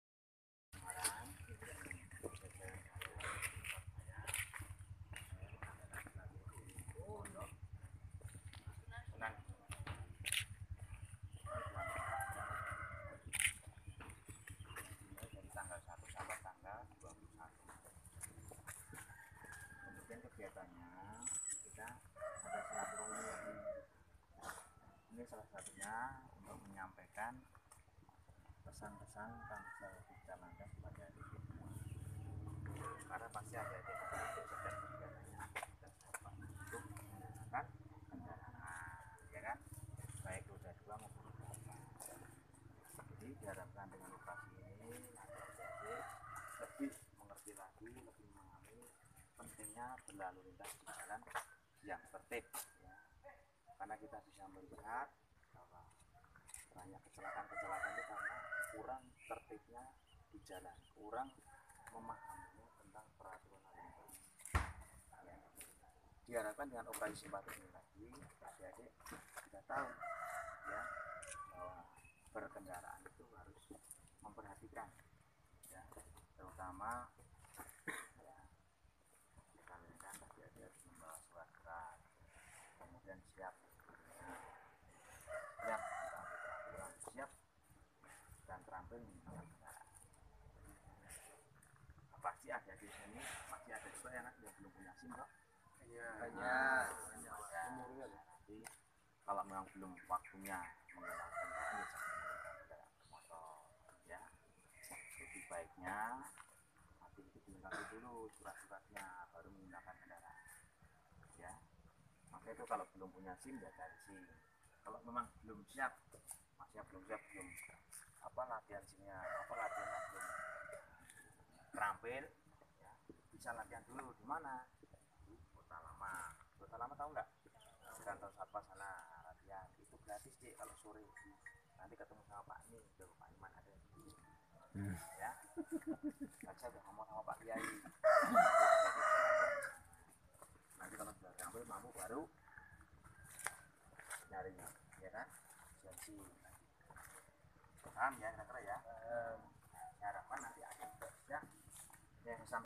Giat Silaturahmi Kamseltibcarlantas di Ponpes Ba'alawy smg terkait Ops Simpatik Candi 2017
Video dari agussaibumi